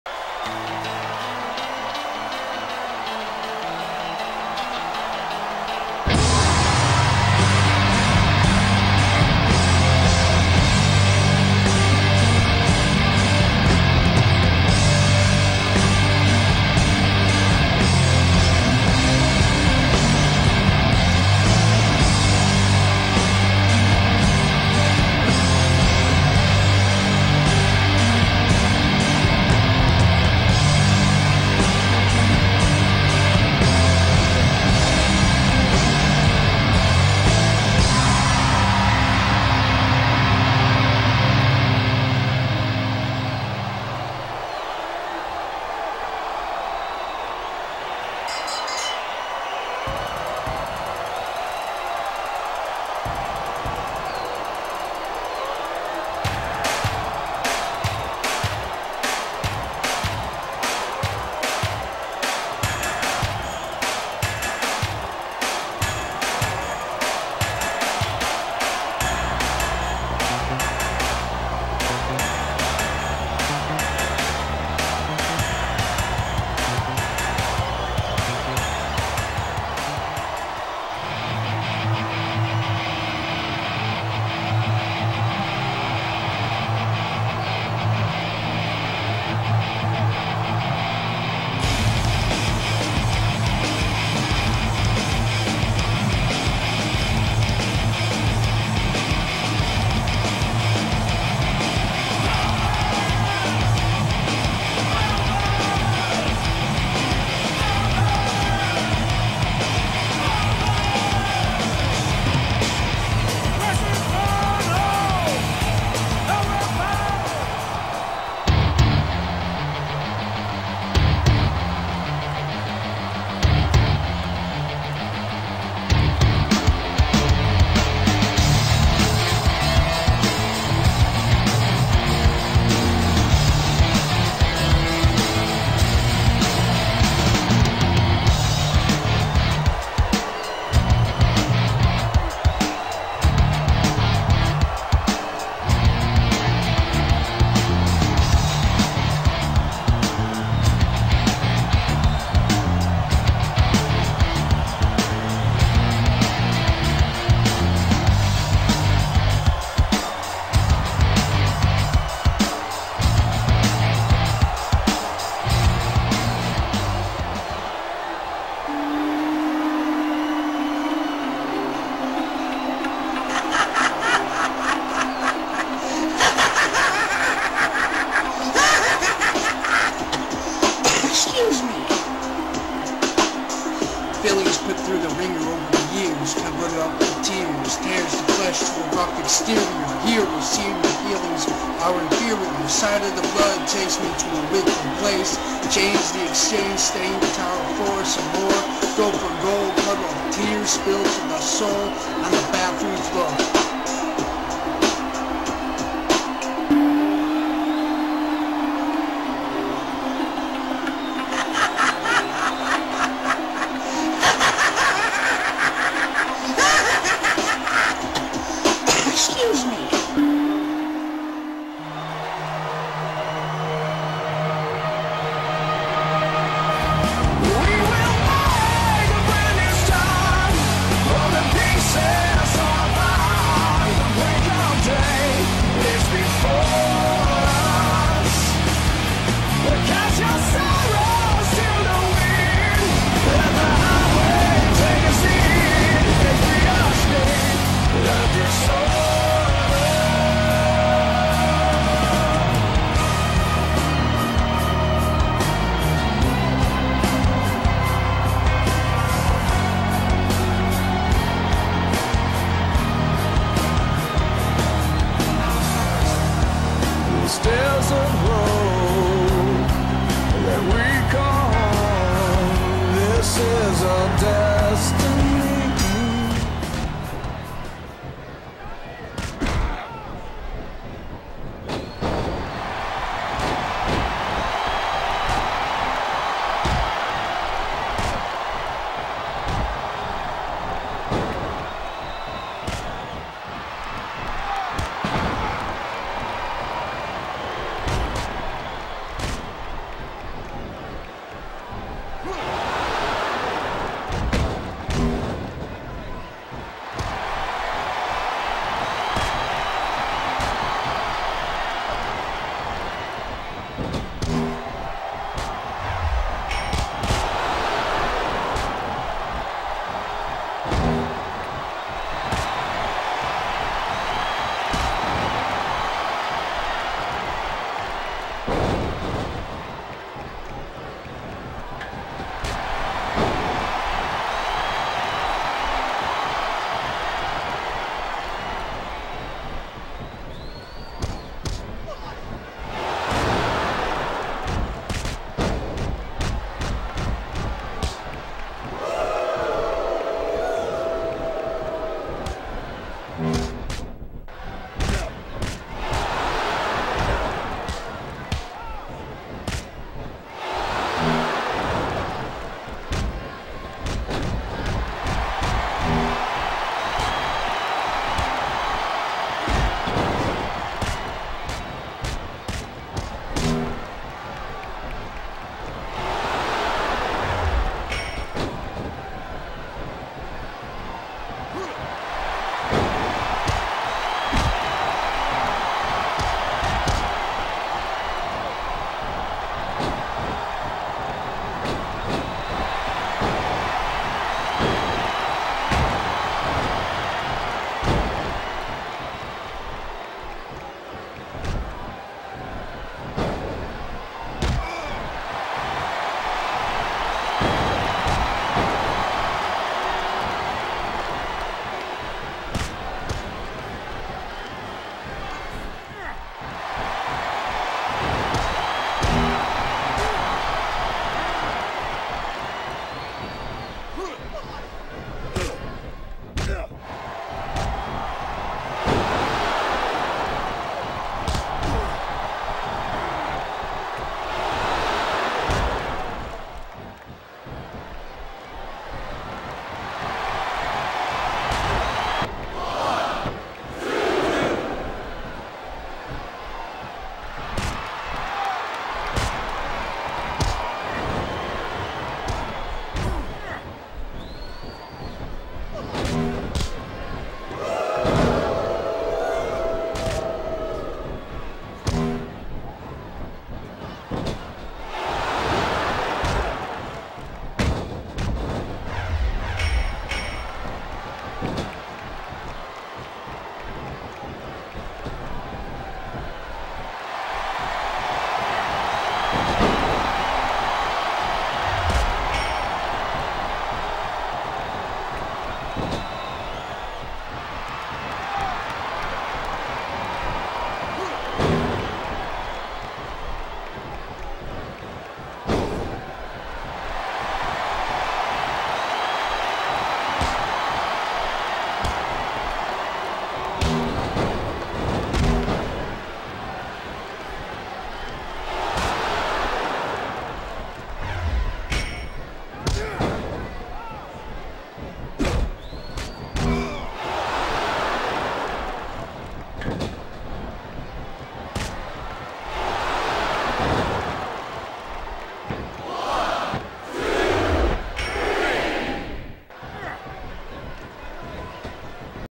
Jobber Belt Jobber Knocker Pt 01 - Feb 12 2009
BlackStone vs Steven Rays vs Dave vs the One vs Slacker vs KeeYa (the reigning jobber champion!)
First one pinned wins Jobber Championship, and the Last Man Standing wins the the chance to hand-pick an opponent to face in a non-title match at a later date, TBD.